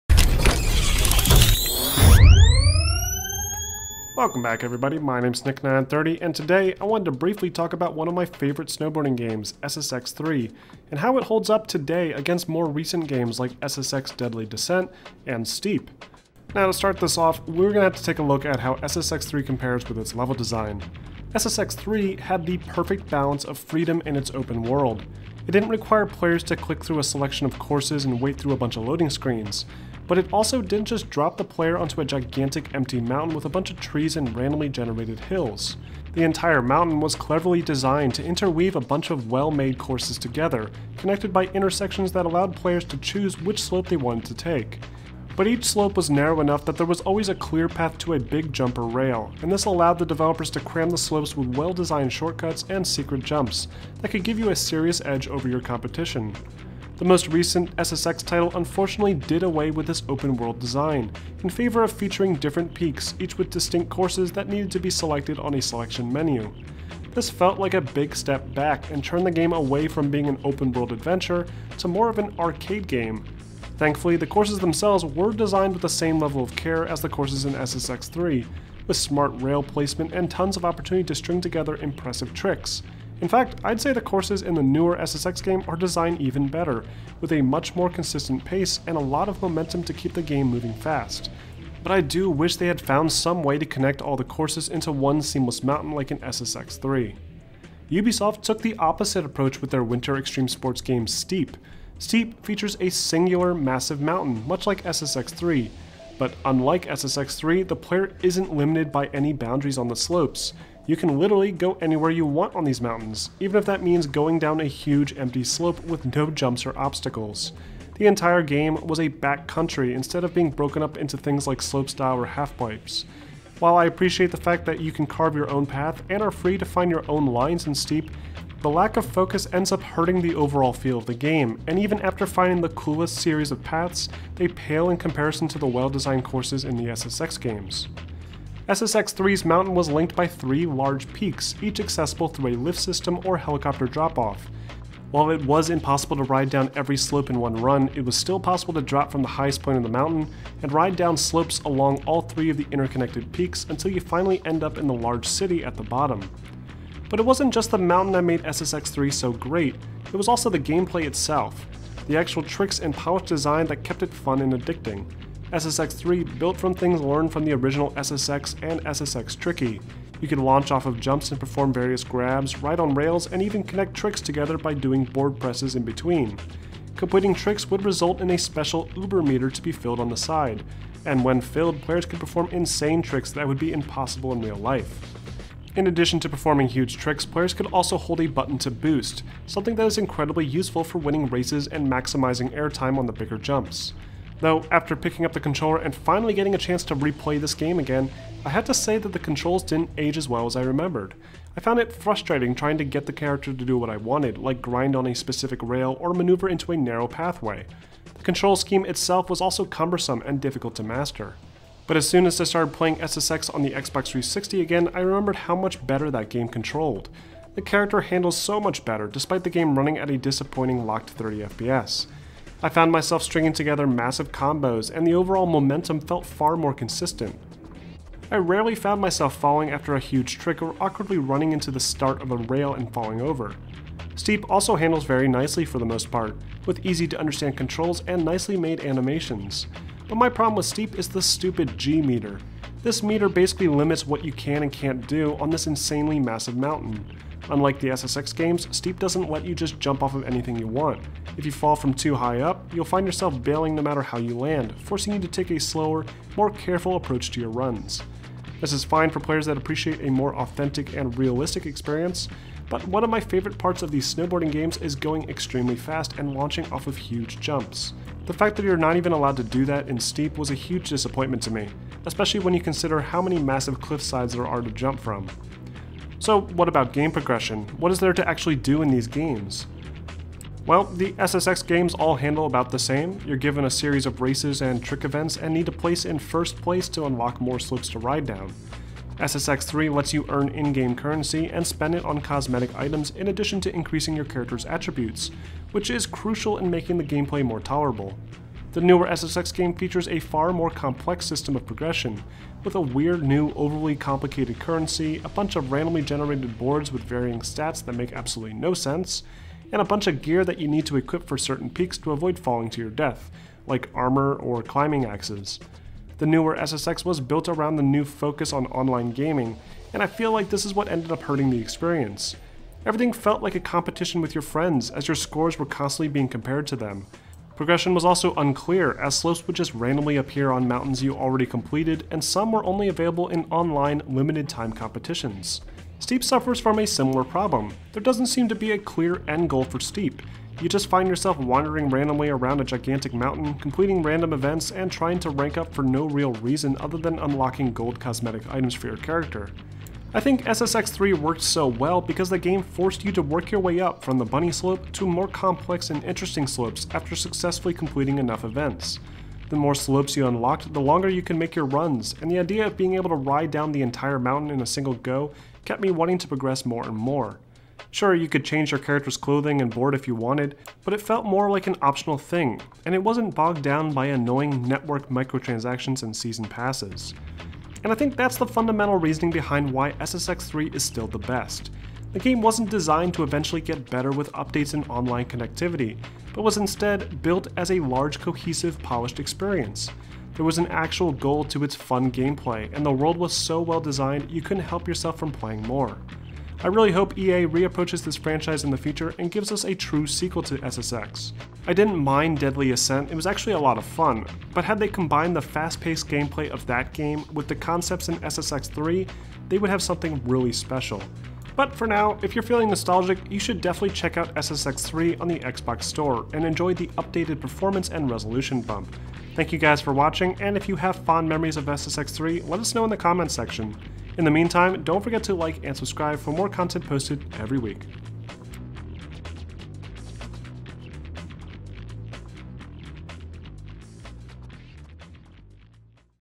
SSX 3 | Retro Review
This is a review I made for SSX 3 back when it was made available on the XboxOne X via backwards compatibility. It's been sitting on my hard-drive for a rainy day (still has an old end-screen too).

Definitely one of my favorite snowboarding games. You guys ever get a chance to play this one?